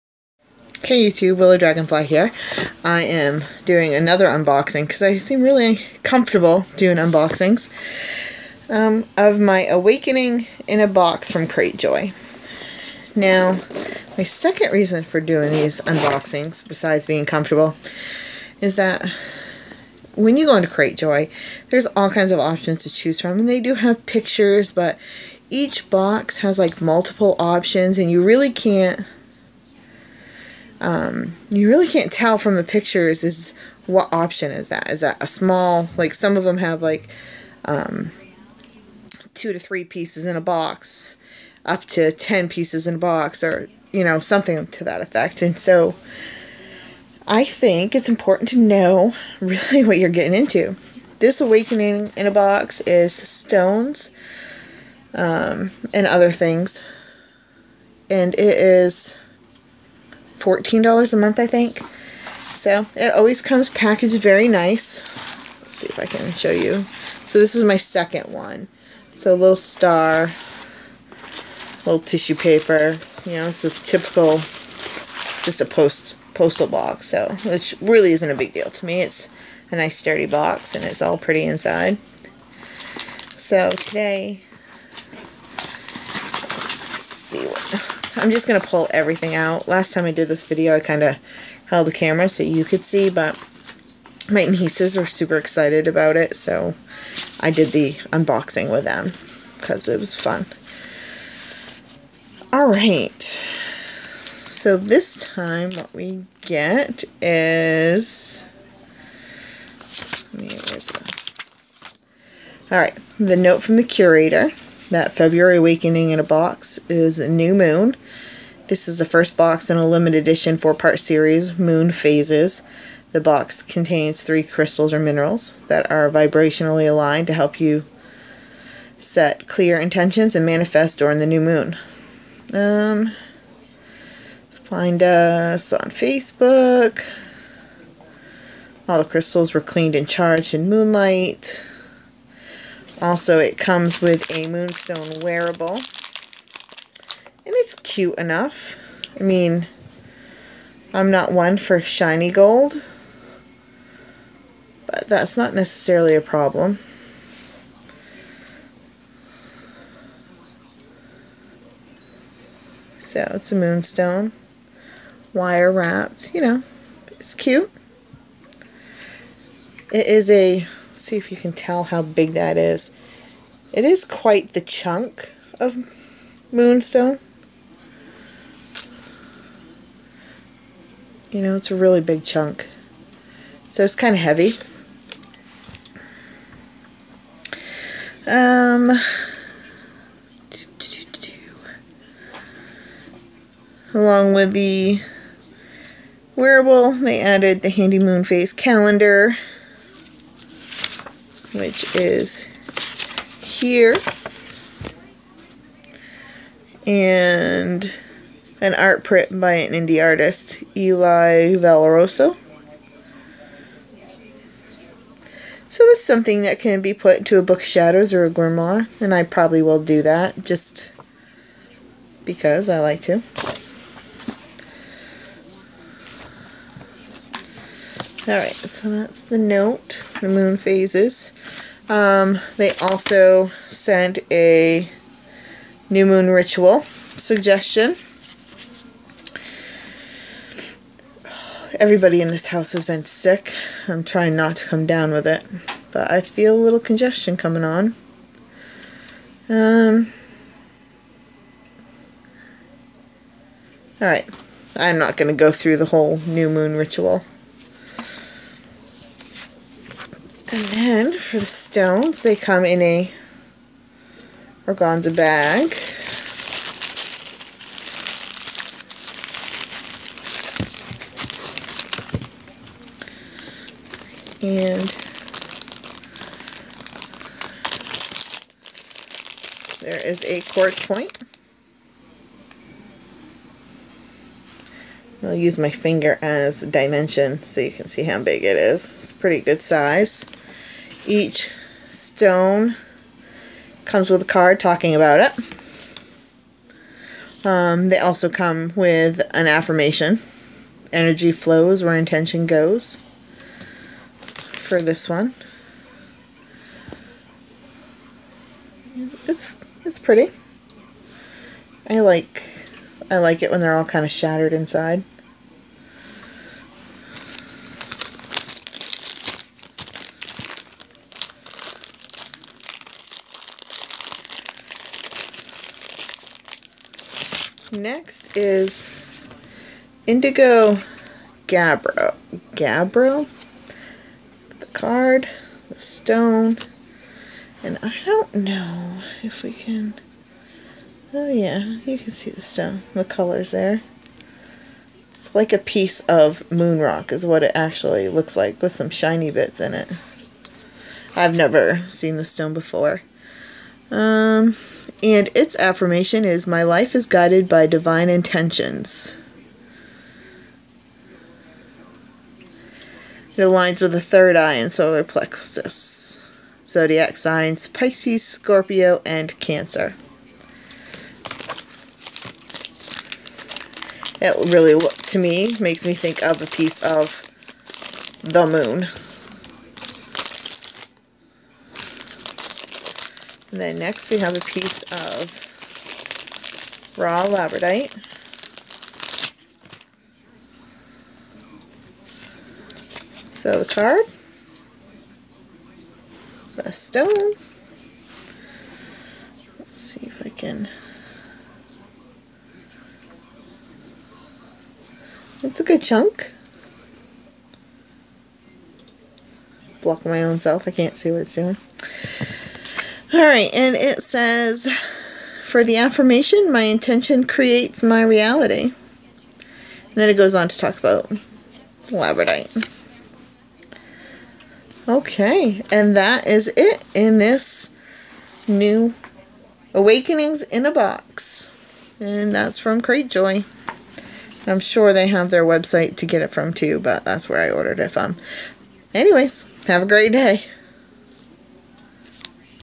Awakening in a box
Hopefully this helps anyone looking to purchase this box from cratejoy.com